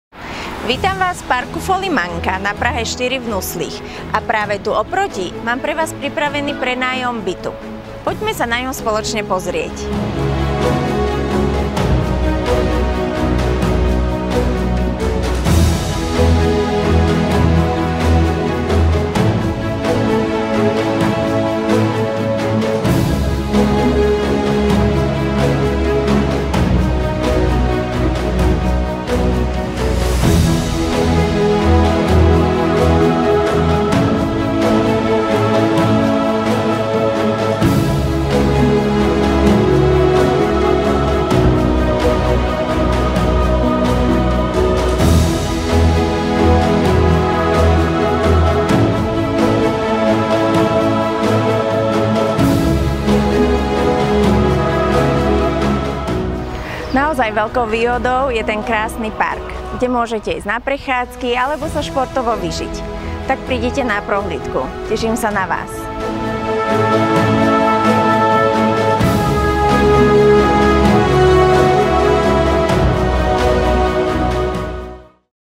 Videoprohlídka - Pronájem 2+1, Praha 2 - Vinohrady
PRONAJATO
Nabízíme k pronájmu kvalitně zrekonstruovaný byt 2+1, 63m2, na Praze 2 - Vinohradech. Byt se nachází ve sníženém 1. nadzemním podlaží. V bytě je jídelna s kuchyňským koutem vybaveným moderní kuchyňskou linkou, 2 velké pokoje, koupelna s prostorným sprchovým koutem a pračkou a samostatné WC. Kuchyňská linka je vybavena spotřebiči - myčka, trouba, elektrická plotýnka a velká lednice. Na oknech jsou vnitřní žaluzie, sítě a bezpečnostní mříže. Byt se pronajímá zařízený. Výhodou tohoto bytu je, že je jediný na poschodí. Bydlení s výbornou dostupností do centra - tramvajová zastávka je poblíž domu. Za domem začíná krásný park Folimanka, který nabízí příjemné procházky. Byt je vhodný také pro spolubydlení.